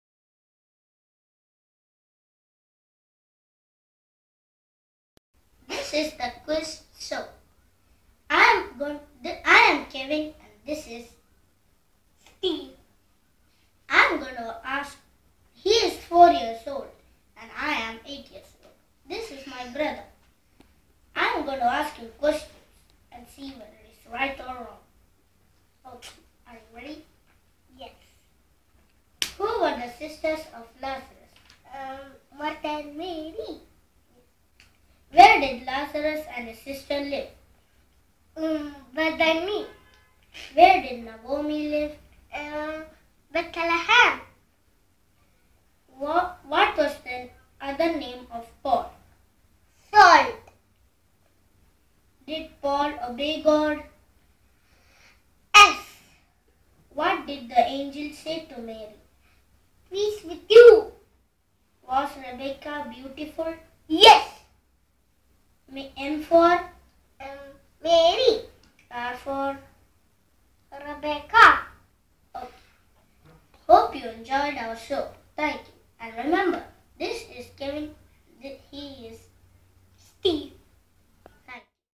STEVE AND KEVIN
Steve's Preparation for his first Sunday School Exam...Quiz master Kevin